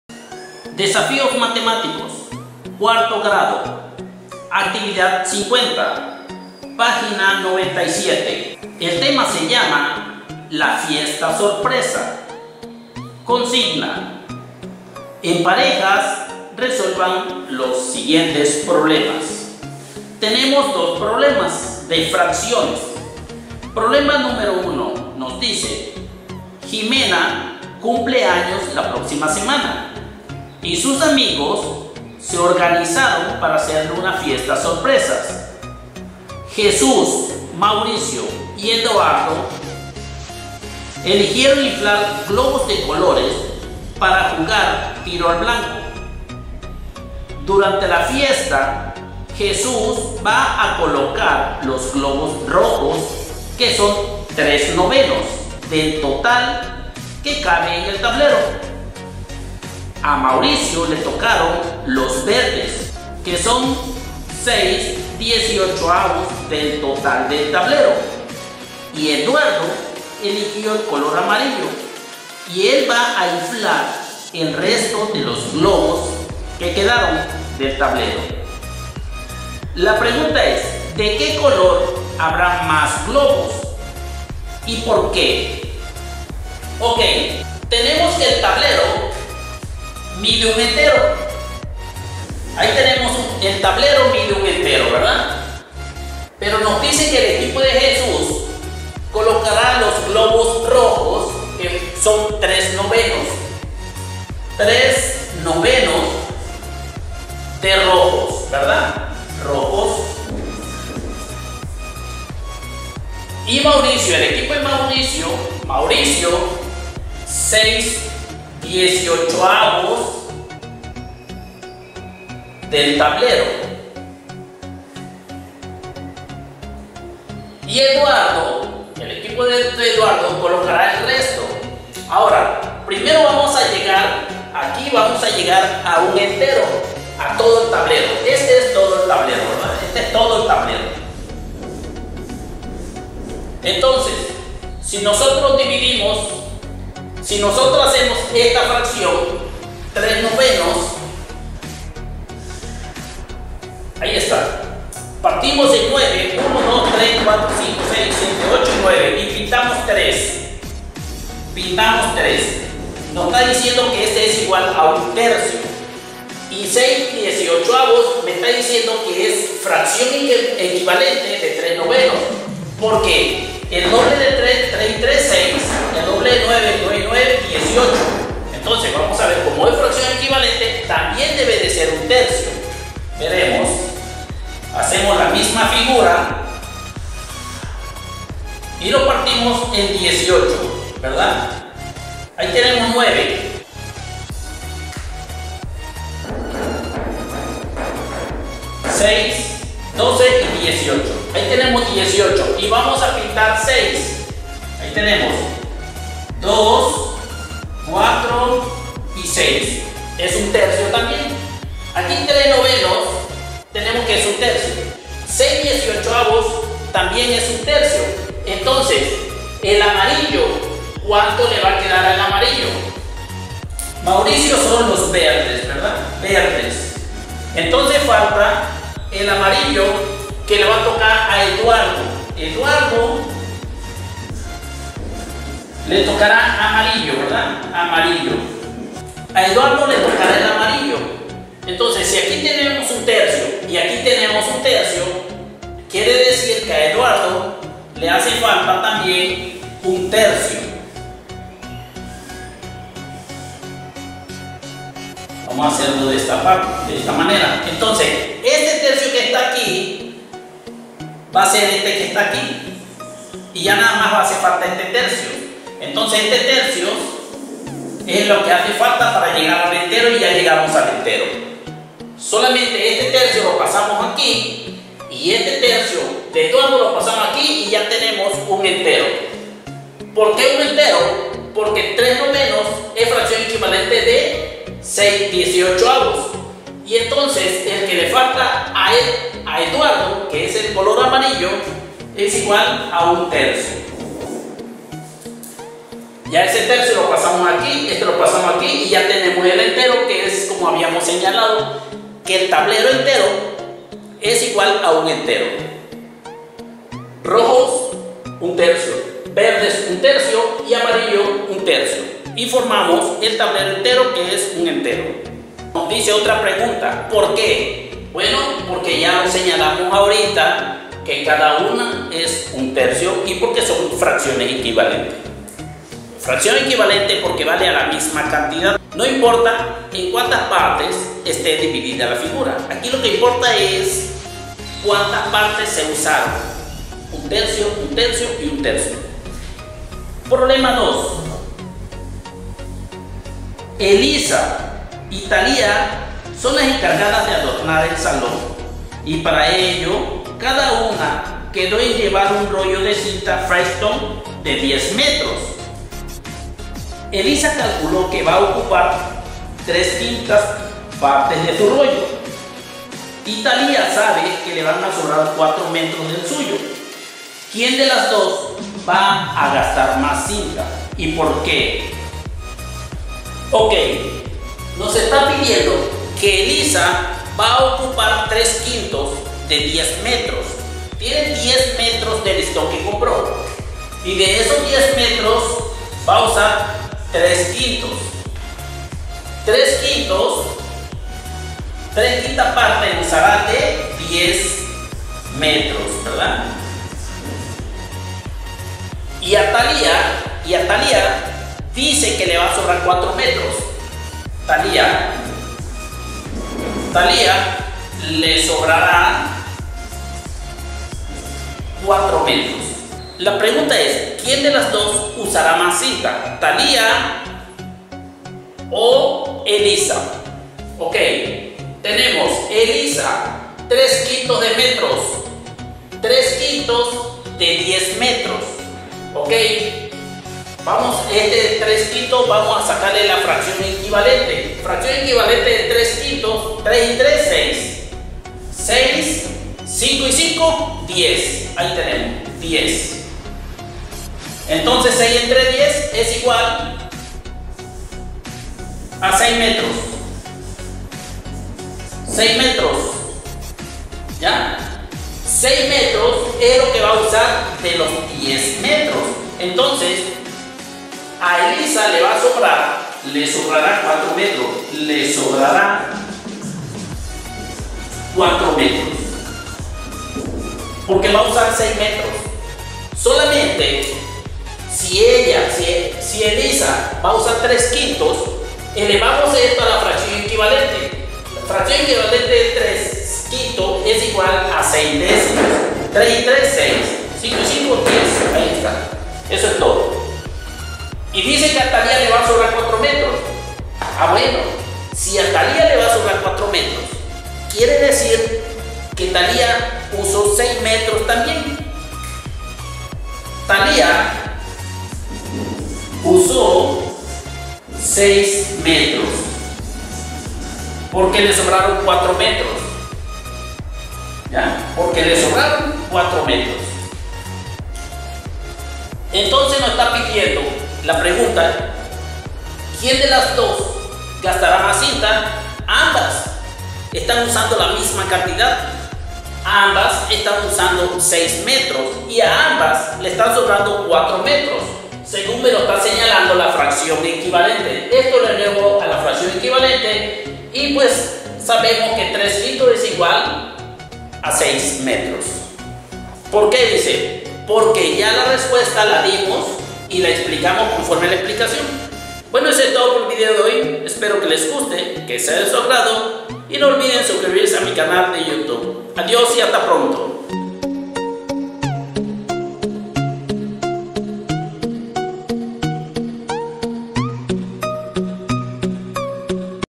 DESAFIOS MATEMÁTICOS 4o GRADO PAG. 97 TEMA. La fiesta sorpresa PROBLEMAS DE RAZONAMIENTO MATEMÁTICO
¡Hola! el PROFE SAMI les ofrece en este video las mejores estrategias para trabajar con problemas de razonamiento matemático involucrando las fracciones equivalentes, sumas y restas de fracciones y el calculo. Les invito a que se aventuren en este material educativo que les puede ser de mucha utilidad. saludos y bendiciones.